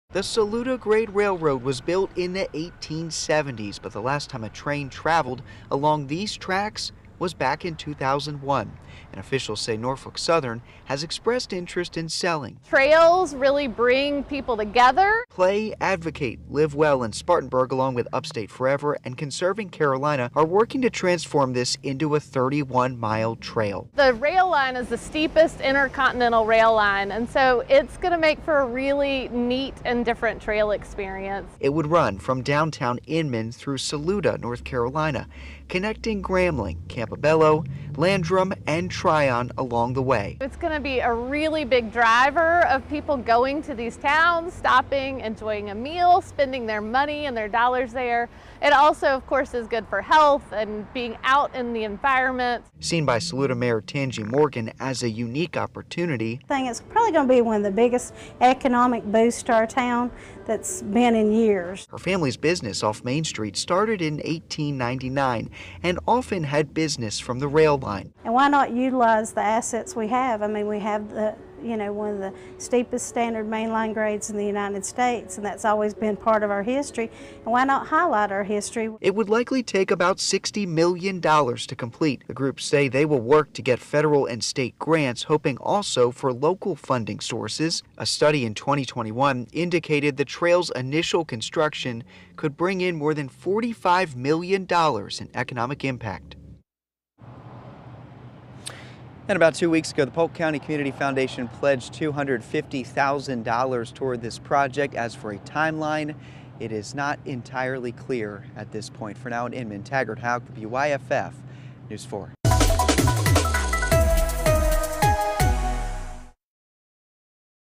Groups work to bring 31-mile trail to Saluda Grade Railroad
There is an effort to bring a 31-mile trail along the Saluda Grade Rail Line, connecting towns in Spartanburg County to Western North Carolina.